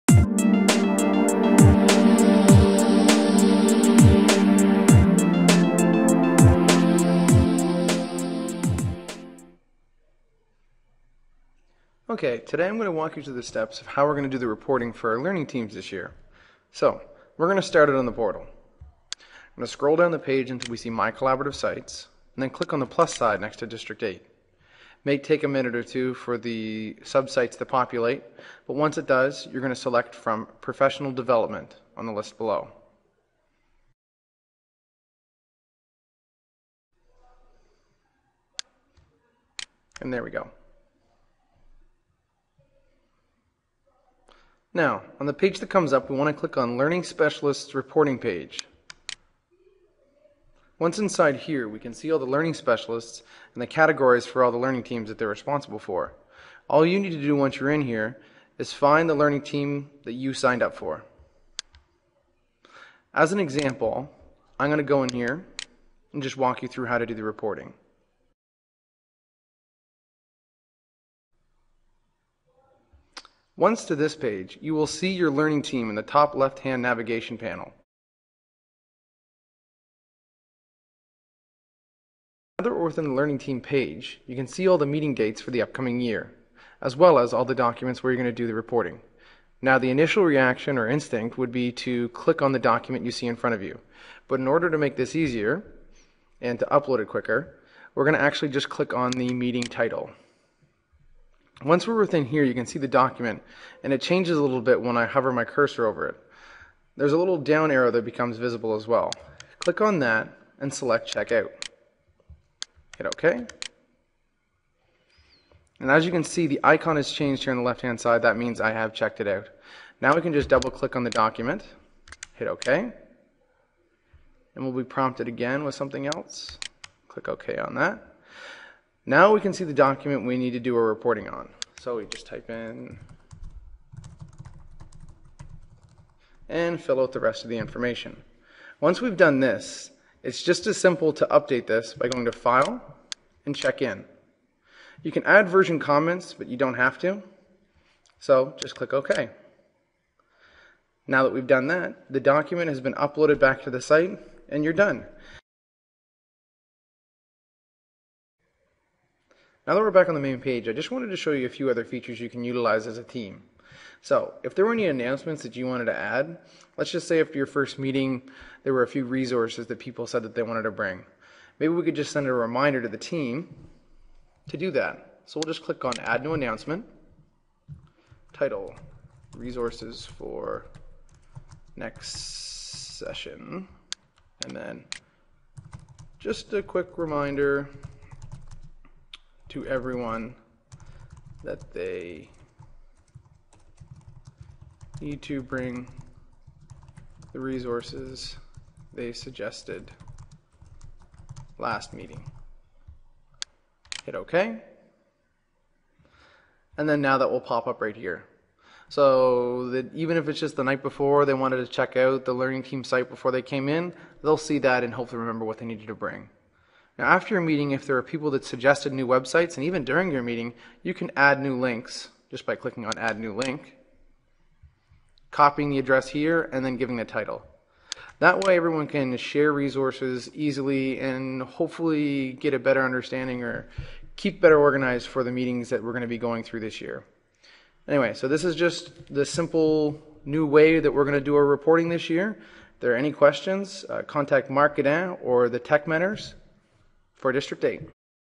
Learning Teams Reporting process - 2011 2012
This demonstration walks through the reporting process for learning teams this year. If there are any questions do not hesitate to ask your levels Technology Mentors.